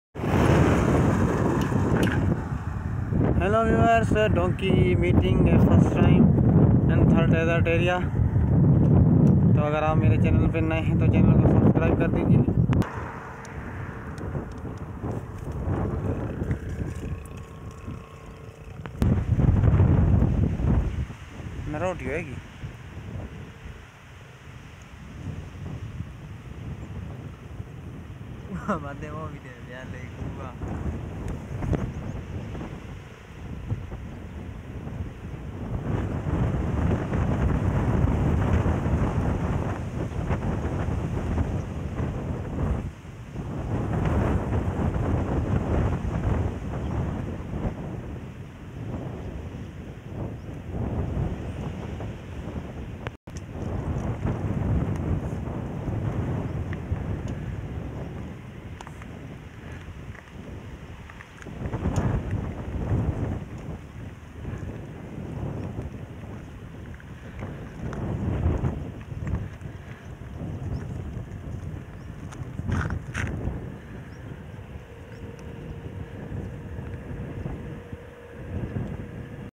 donkey road fighting
The Big madhi Ghadhi Mating season first time fighting donky video male and female donkey meeting